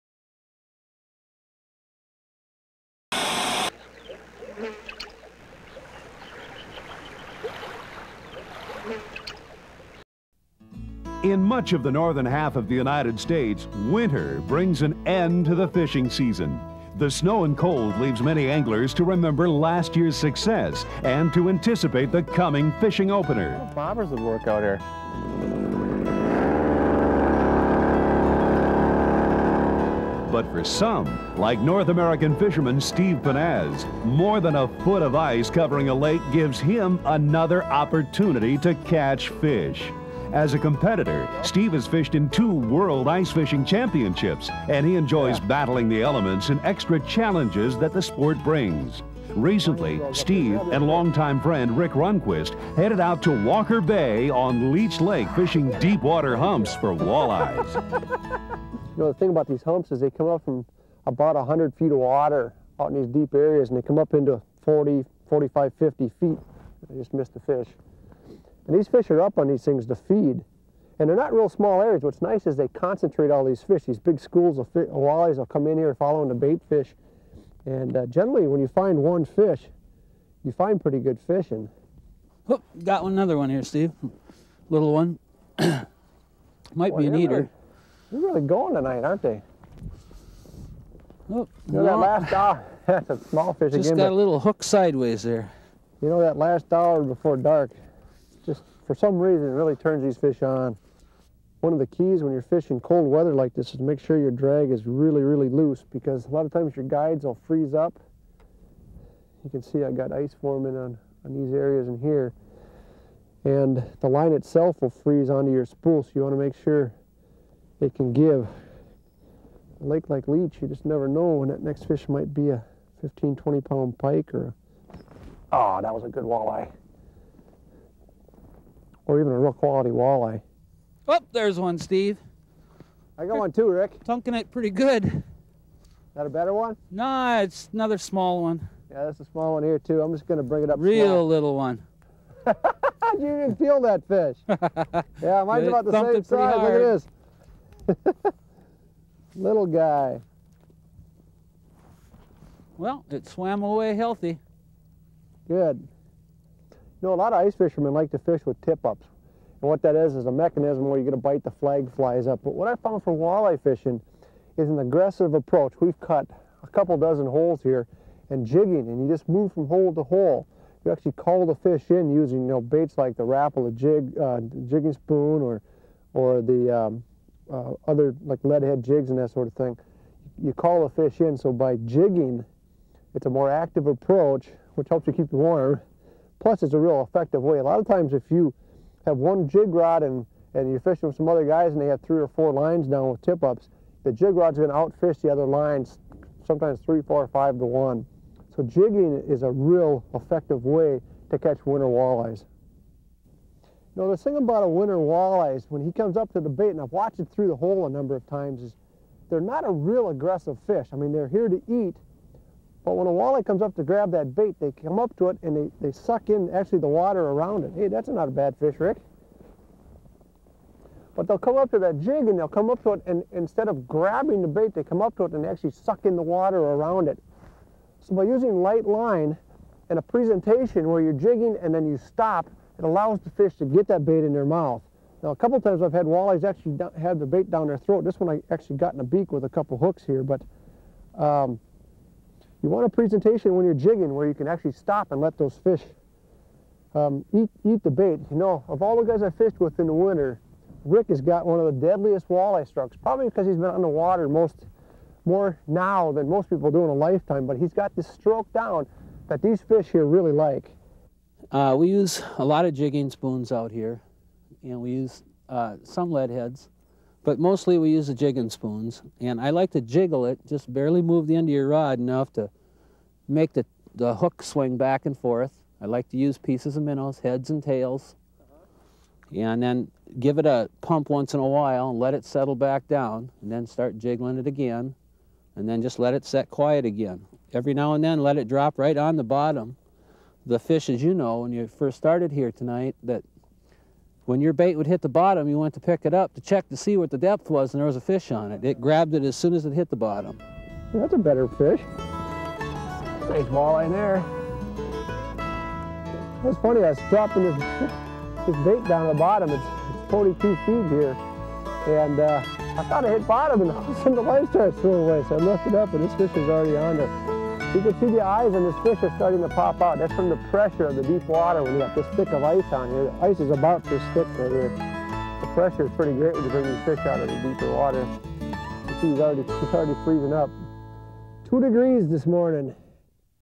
North American Fishing Club: Fishing Tips From the Walleye Pros. Part 8
Learn the best jig, bait and line fishing techniques for catching a walleye ice fishing. Video presented by the North American Fishing Club.

Like this video? We think you'd "like" our Facebook page too. Stay up to date on all things FishingClub